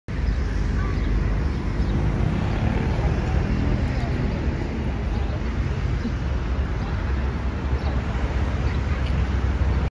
Flower clock in Geneva, Switzerland
Colorful flowerclock in the heart of geneva switzerland 🇨🇭
Created in 1955 with a diameter of 5 meters, until 2005 was the largest flower clock in the world.
More than 6.500 flowering plants are used for the clock face and it looks different every time, depending on the time of year you are visiting

Zivopisan cvetni sat u srcu Zeneve, Svajcarska 🇨🇭
Napravljen 1955. godine, sve do 2005. bio je najveci cvetni sat na svetu.
Vise od 6.500 cvetnih biljaka formira izgled sata koji se moze drasticno razlikovati svaki put kada posetite Zenevu, zavisno od godisnjeg doba vase posete.